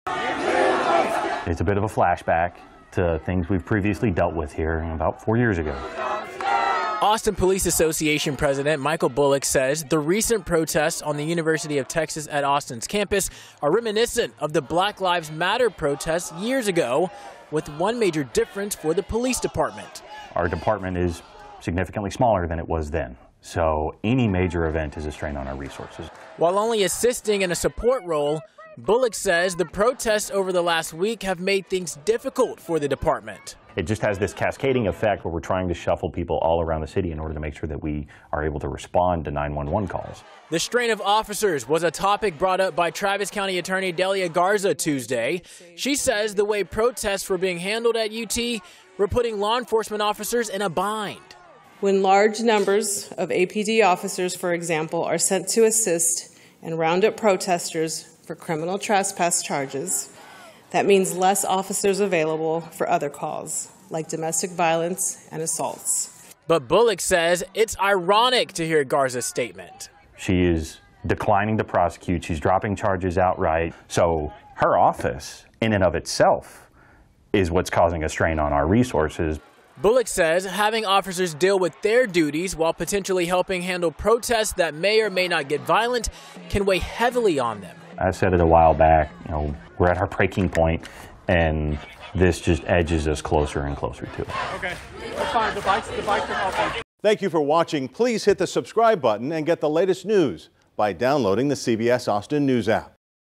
Austin police at breaking point, union president says
According to Austin Police Association President Michael Bullock, the Austin Police Department is currently facing a shortage of officers, and assisting with protests at the University of Texas at Austin this week has added to the strain on the department's resources, leading to officers being pulled from their regular duties, including responding to 911 calls.